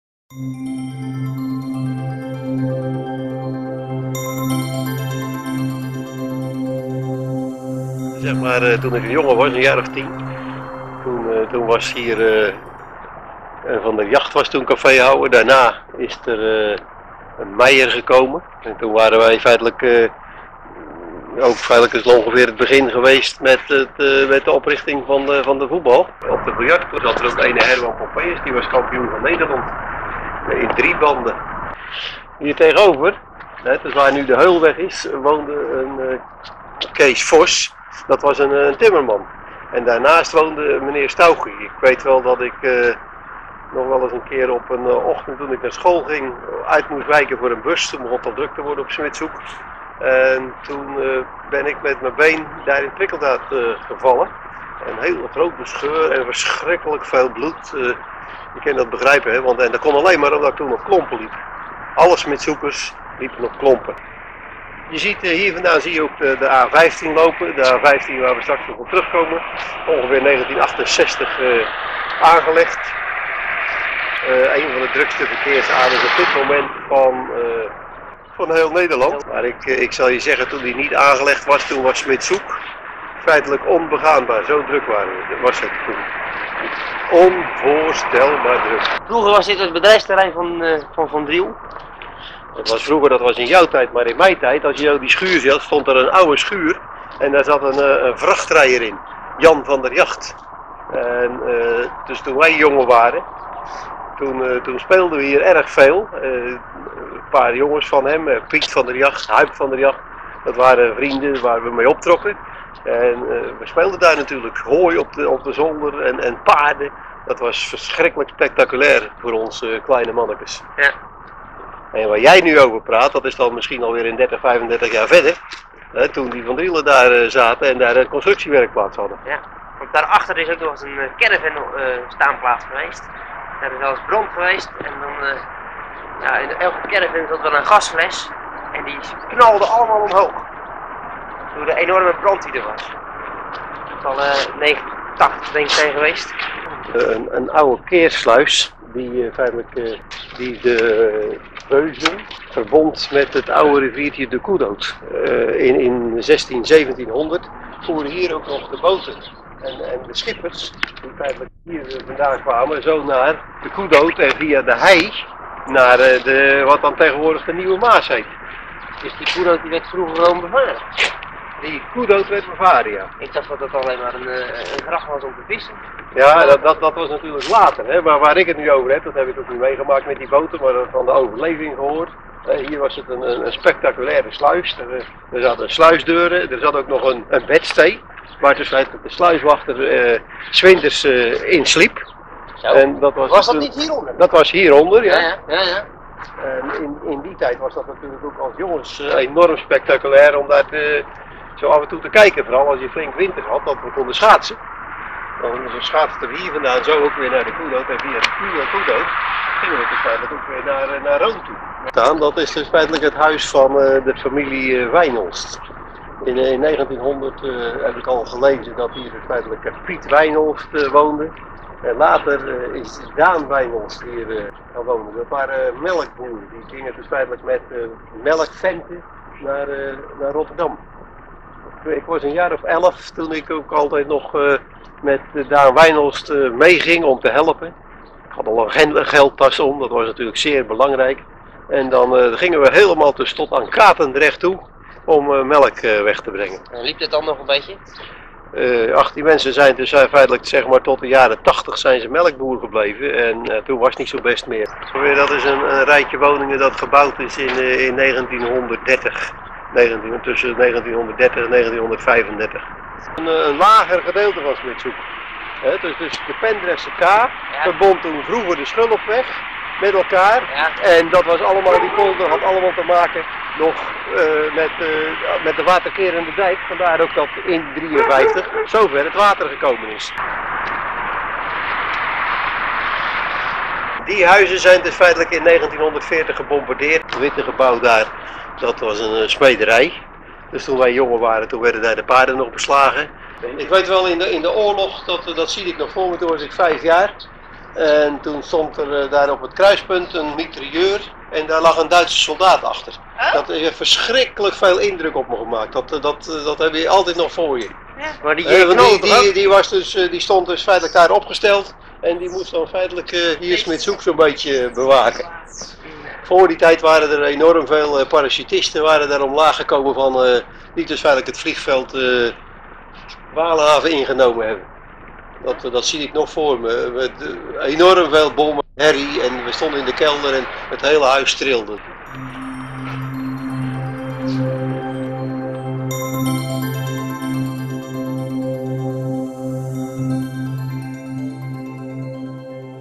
1996 Joop Nootenboom vertelt over het Smitshoek van zijn jeugd.
Smitshoeker Joop Nootenboom vertelt over de kleine buurtschap van Barendrecht.Een film uit de collectie van